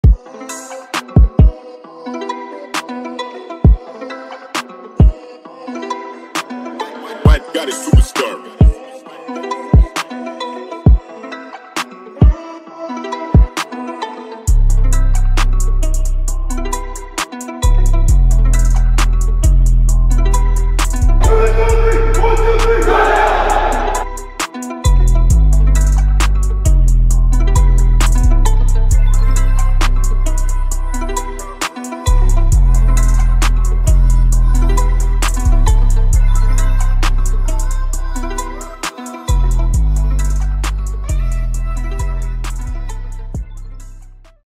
Men's Soccer Promo
Communication Arts student Harrison Askey created this promo video for our men's soccer team.